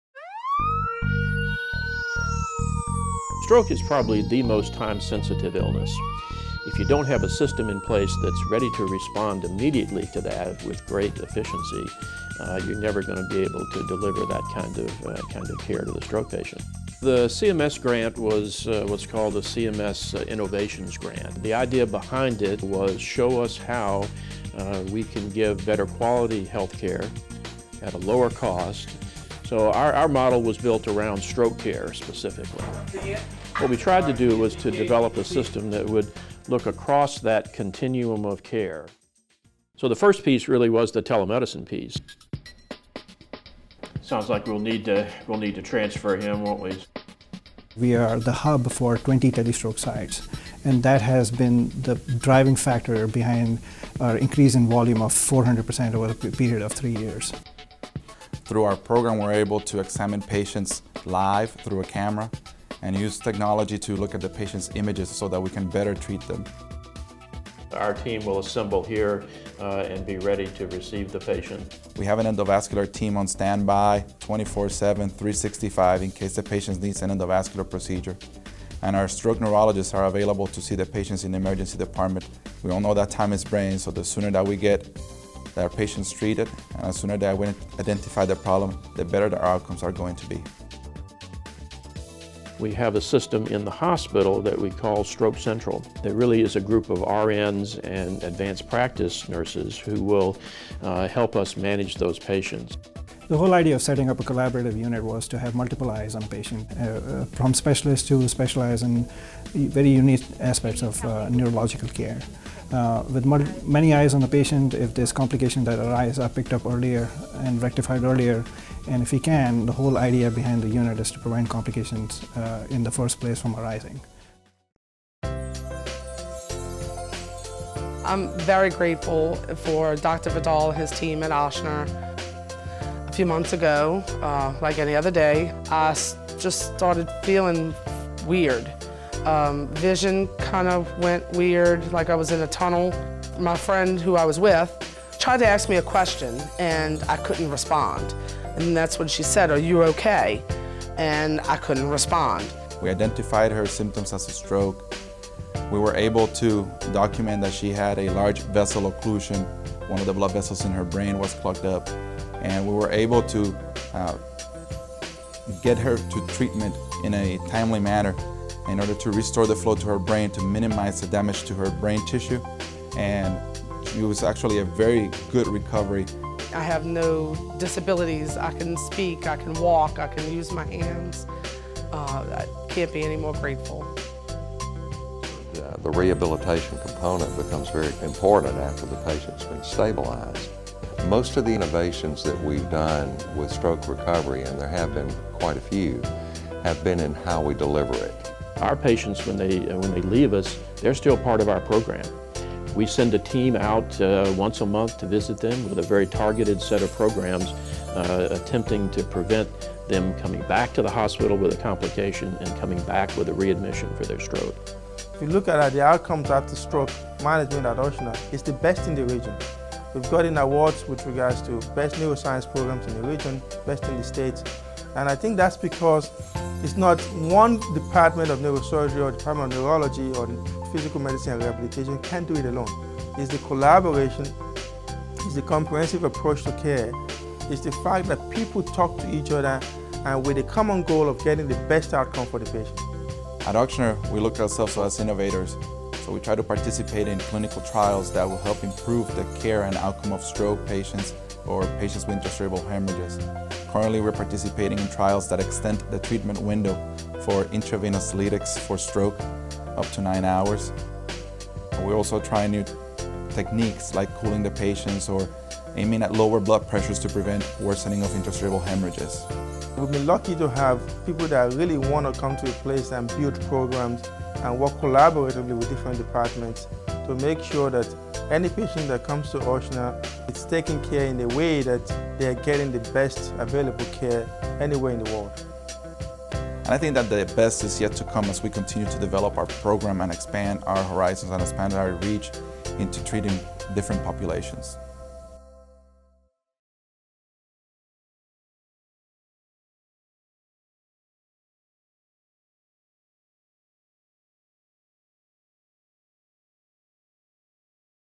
Ochsner Neuroscience Institute - An Innovative Approach to Stroke Care
Physicians at Ochsner Medical Center in New Orleans had the opportunity to get innovative with a 7-million-dollar grant from CMS. They devised a new methodology in delivering stroke care with a goal of providing better quality healthcare at a lower cost. The collaborative, comprehensive approach is already improving outcomes and physicians say the best is yet to come.